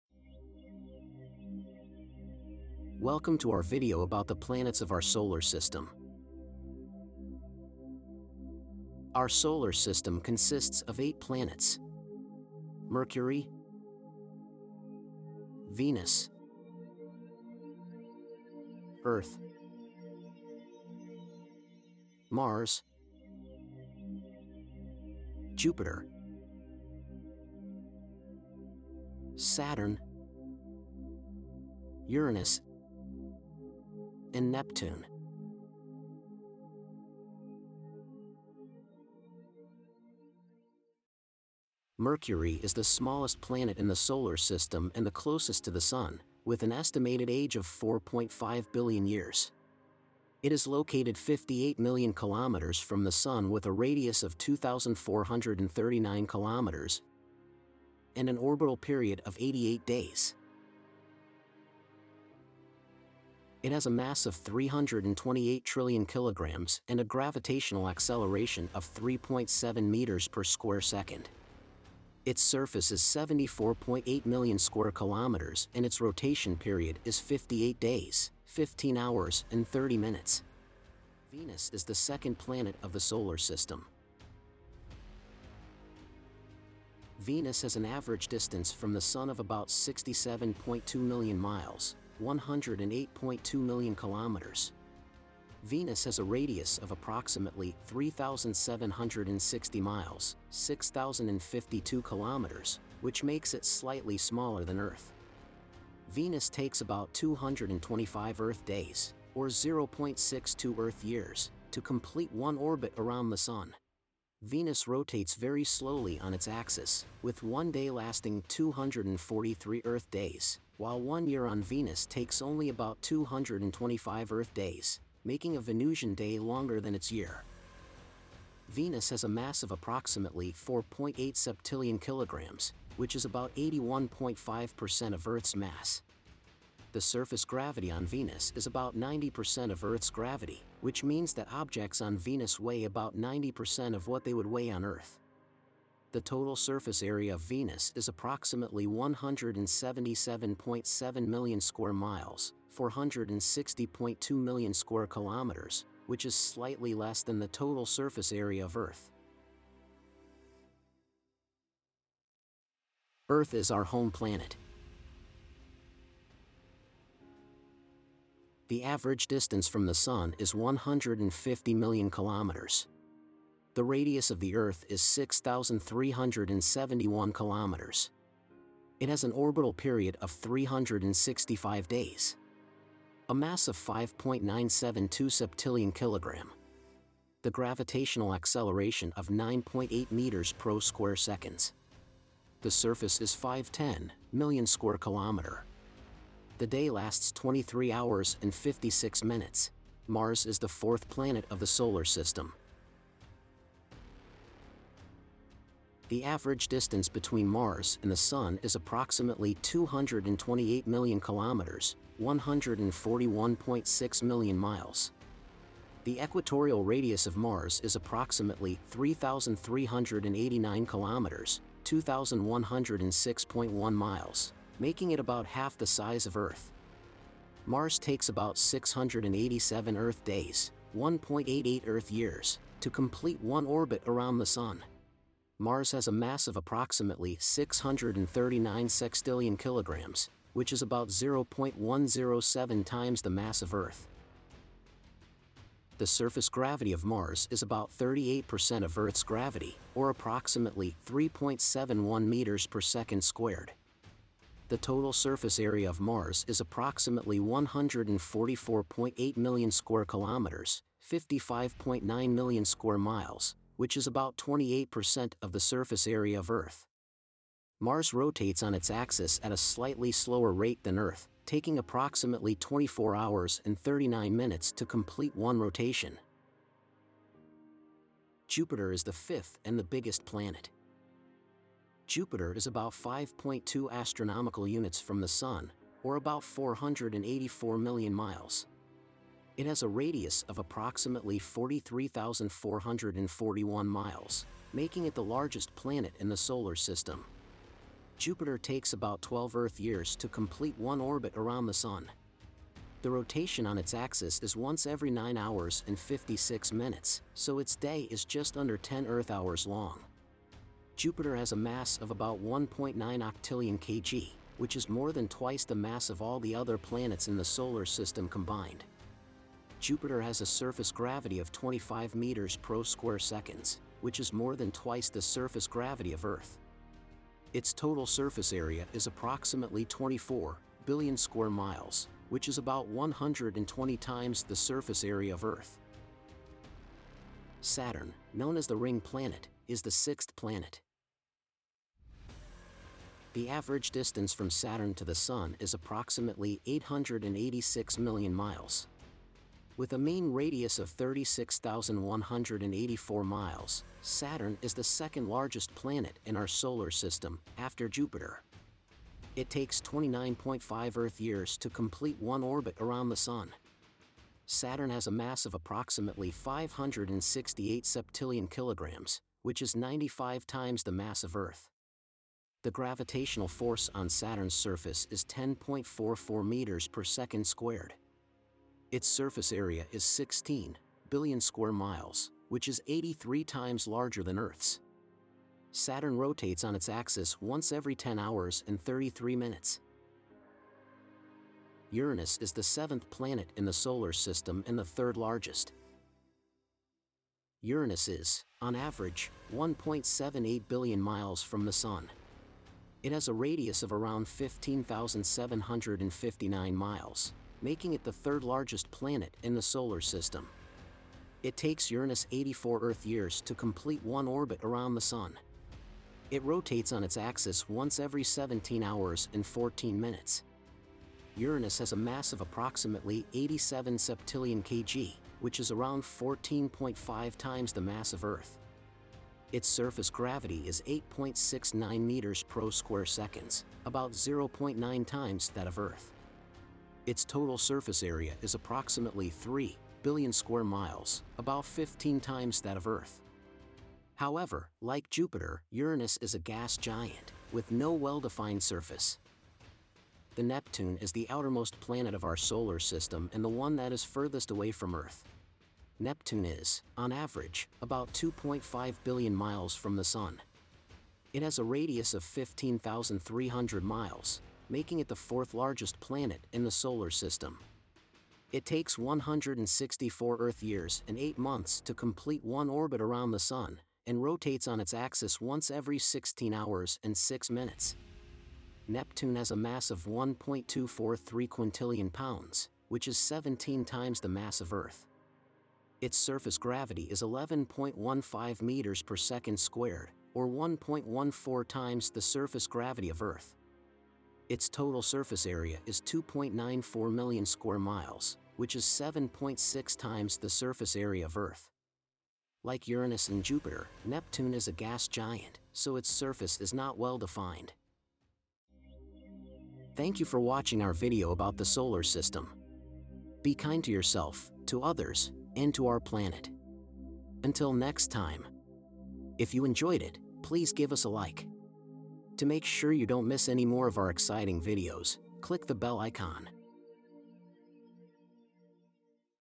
Solar System
Our Solar System is home to eight planets, ranging from Mercury, the closest planet to the Sun, to Neptune, the farthest planet from the Sun. Jupiter, the fifth planet and the largest, is known for its beautiful rings. Saturn, the sixth planet, is also known for its rings. Uranus is the seventh planet and the third largest, while Neptune is the outermost planet and the furthest from Earth. In our video, we discussed the various planets in our Solar System and the fascinating facts about each of them.

00:00 Intro
00:40 Mercury
01:21 Venus
02:42 Earth
03:19 Mars
04:37 Jupiter
05:37 Saturn
06:31 Uranus
07:29 Neptune